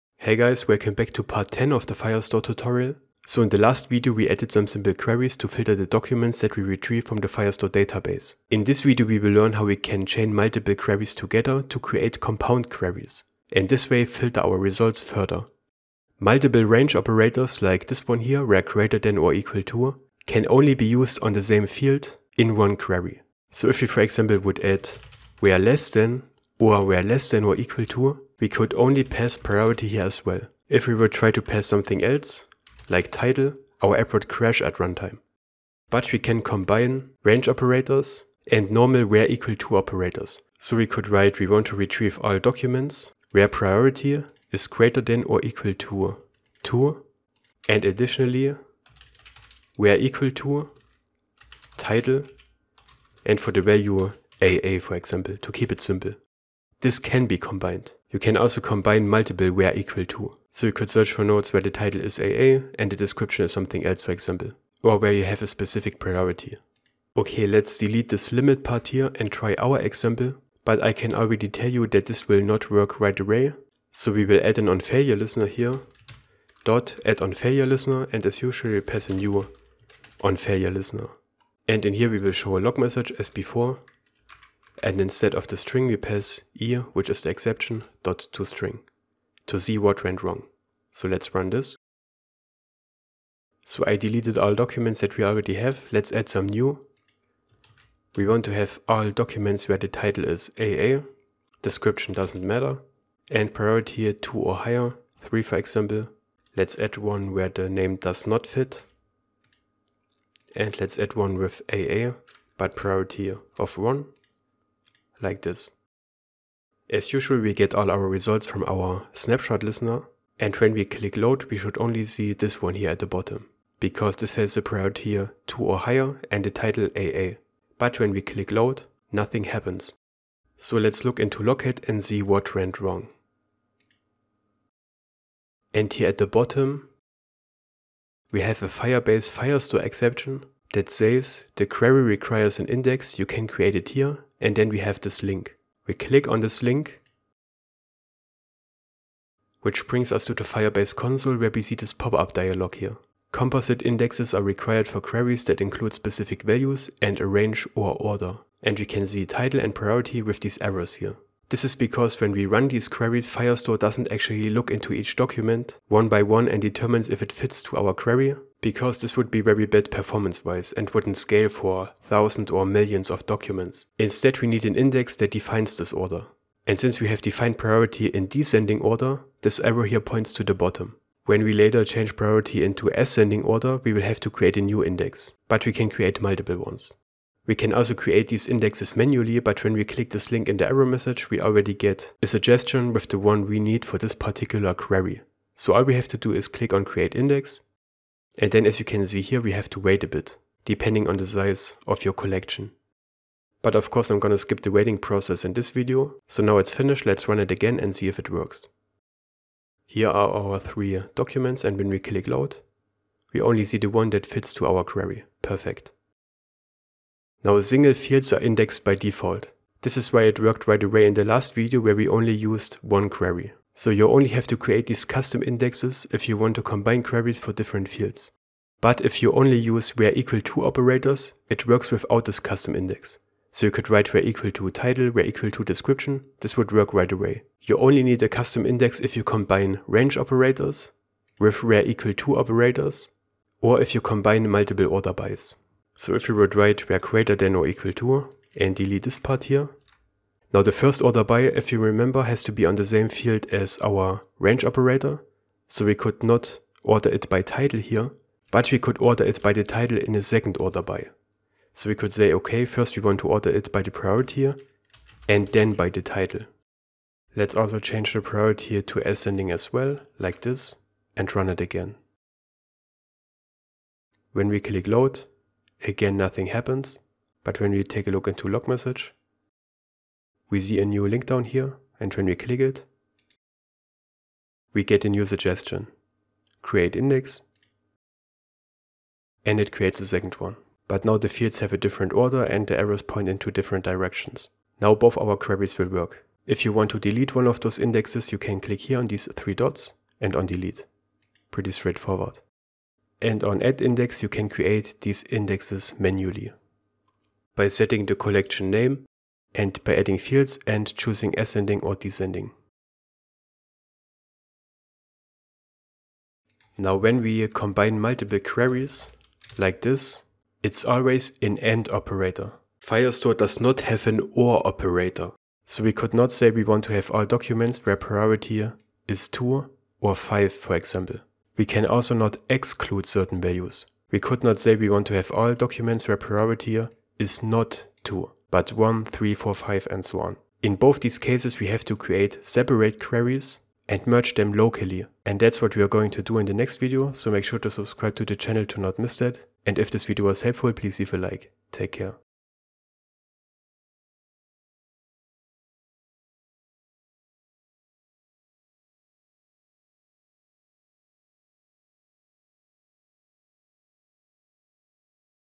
Firestore Tutorial Part 10 - COMPOUND QUERIES - Android Studio Tutorial
In part 10 of the Firestore tutorial we chain multiple queries together to create compound queries and this way filter our results further.
If we want to combine the whereEqualTo operator with range operators like whereGreaterThanOrEqualTo or whereLessThan, or if we want to chain multiple orderBy together, we have to create custom indexes in the Firebase console.